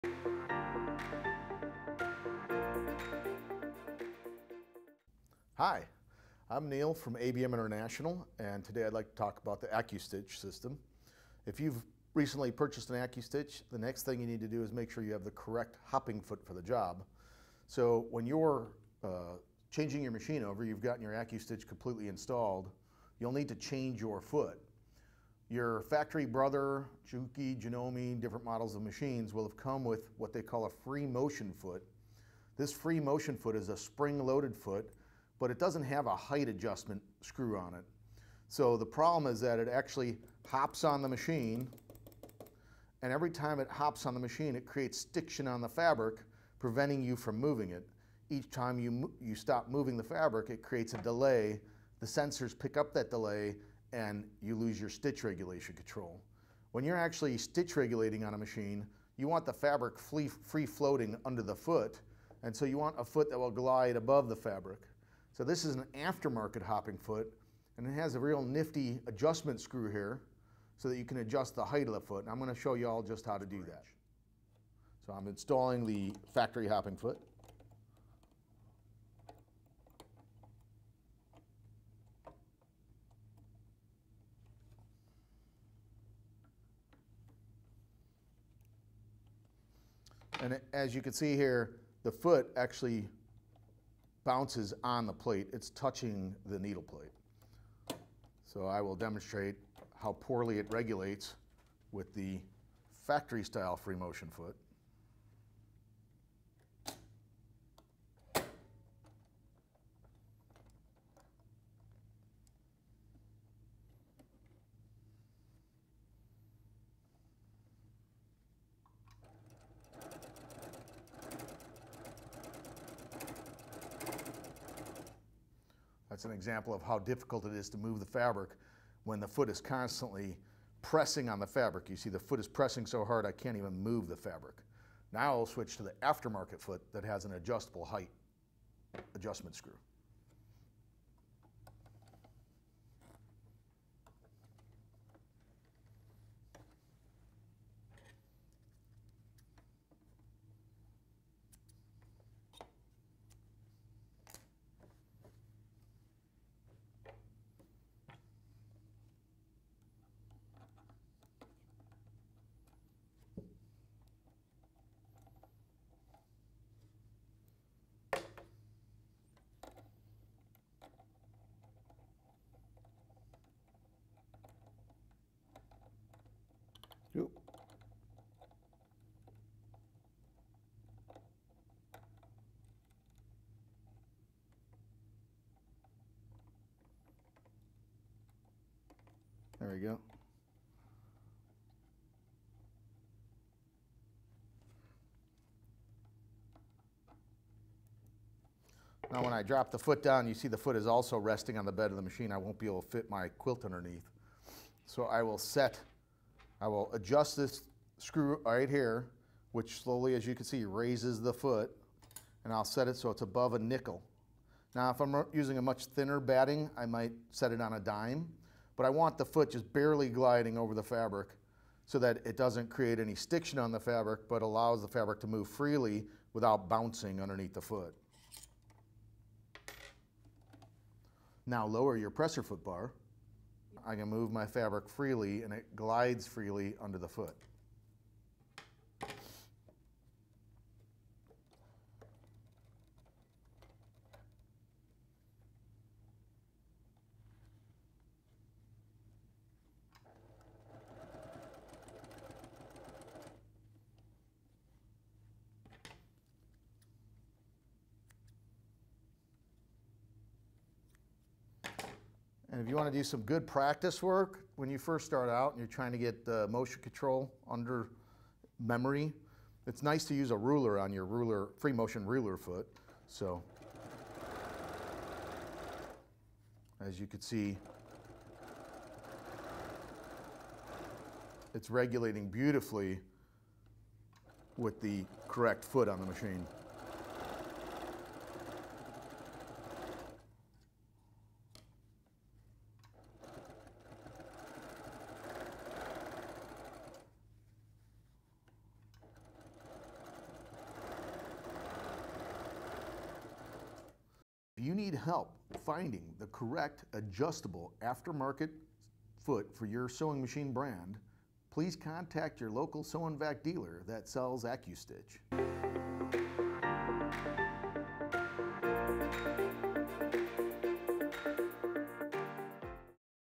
ACCUSTITCH | Choosing the Correct Hopping Foot
After purchasing Accustitch, make sure you have the correct hopping foot.